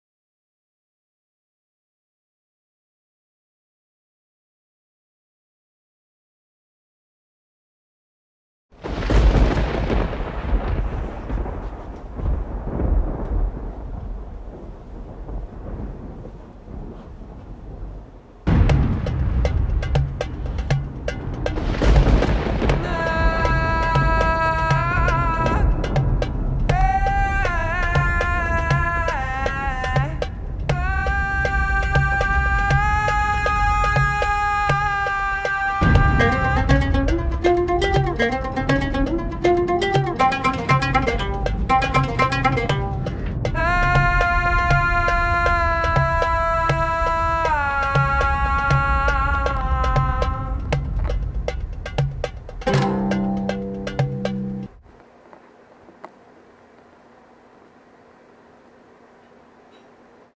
Black Widow
Cool video of a black widow (I had hoped I could record hunting footage).  Song is made by me in GarageBand since YouTube has been sending me copyright violation warnings and stuff. I like it. I hope y'all do.